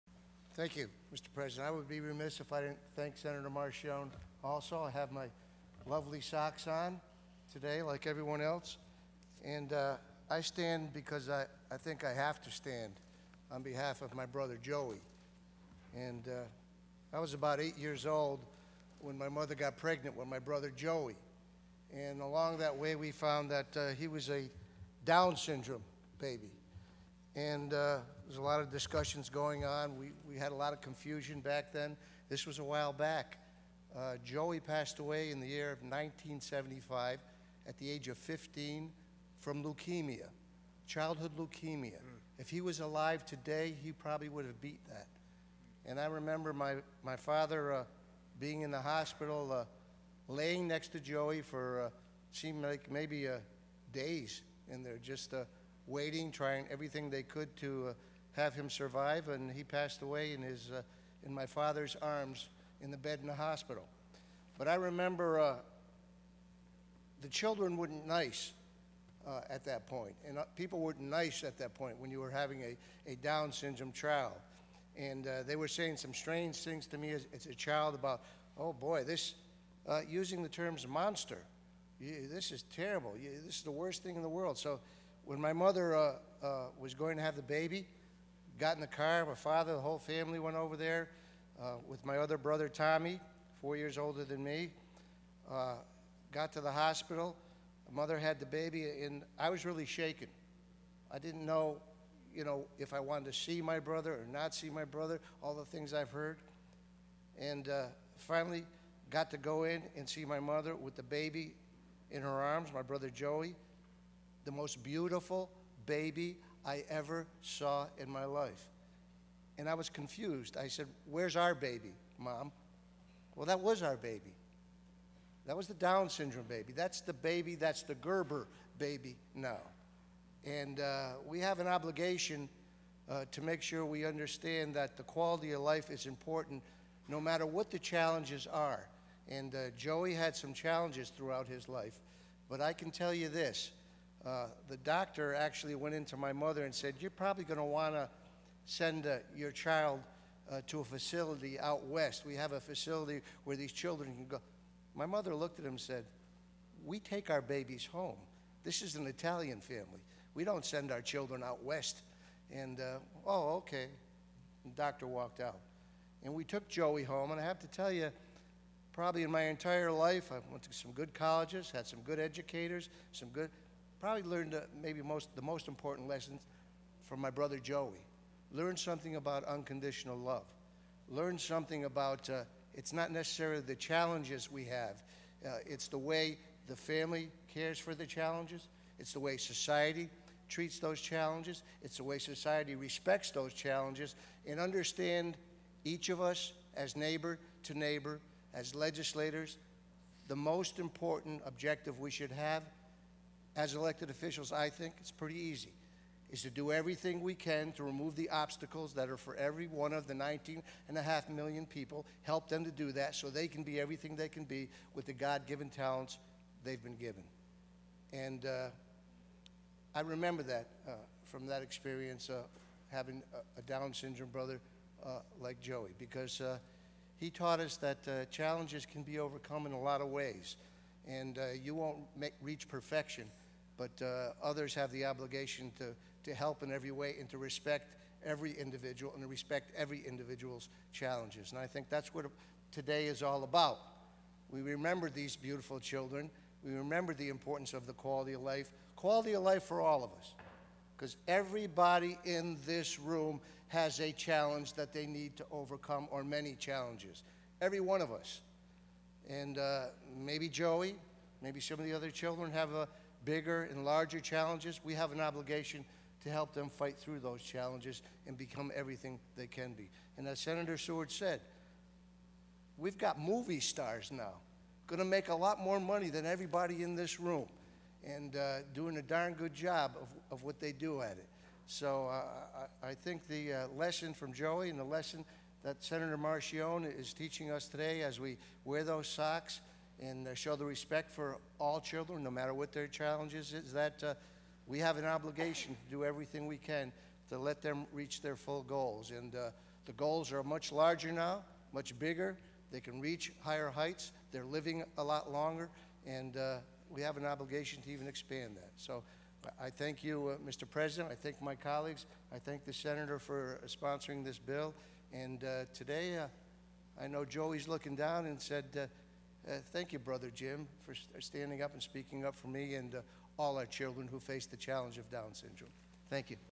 Remembering My Brother Joey Tedisco on Down Syndrome Awareness Day
I had to stand today on the NYS Senate Floor to be a voice for and tell a very personal story about my late brother, Joey Tedisco, who had Down Syndrome.  Today is Down Syndrome Awareness Day and we passed a resolution sponsored by Senator Kathy Marchione, which I co-sponsored to bring attention to our family, friends, neighbors and fellow citizens who have Down Syndrome.  We have an obligation to do everything we can to let them -- and all New Yorkers -- be everything they can be with the God-given talents they've been given.